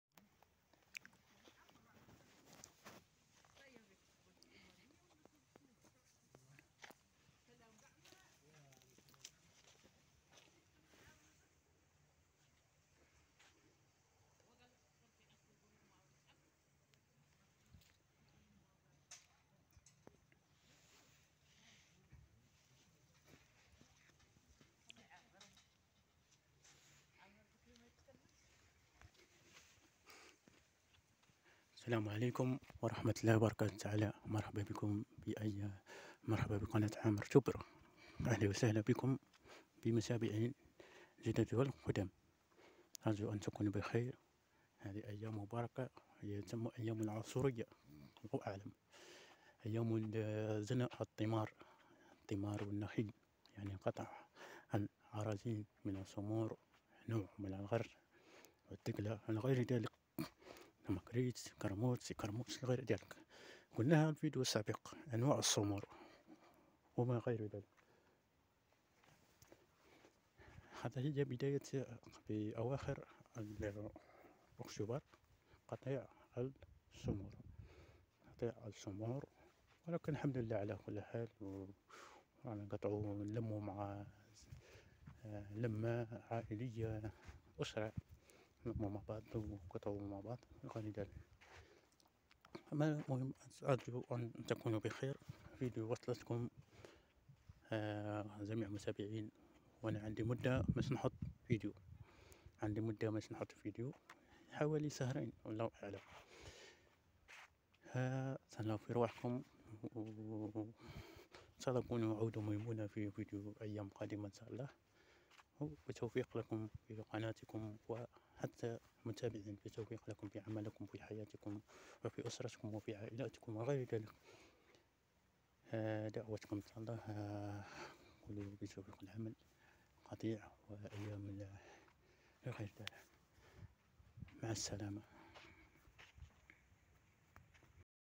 انطلاق جني تمور و انواع تمور ولاية ورقلة بسم الله ماشاء الله قناة عامر سوبر
سلام عليكم ورحمة الله وبركاته
انطلاق جني تمور ولاية ورقلة
بسم الله ماشاء الله
اللهم صلي وسلم وبارك على سيدنا محمد